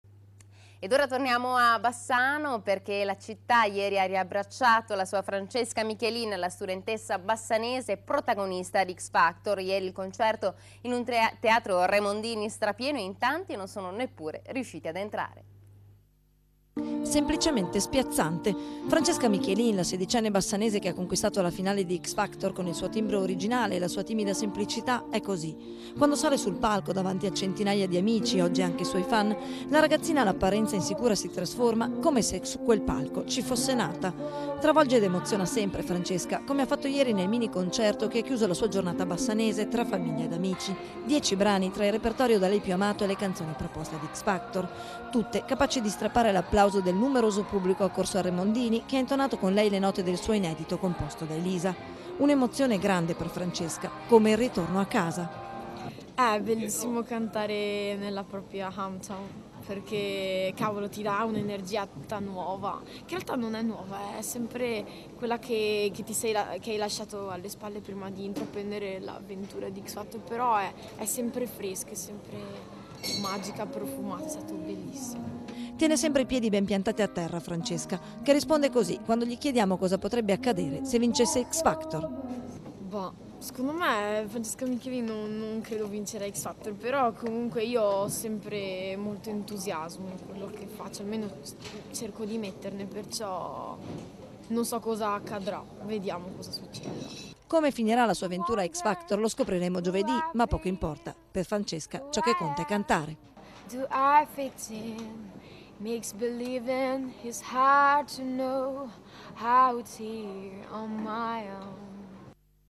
Francesca Michielin X Factor 5 concerto a Bassano del Grappa
Servizio del tg Bassano di Rete Veneta sul concerto di Francesca Michielin finalista di X Factor 5 a Bassano del Grappa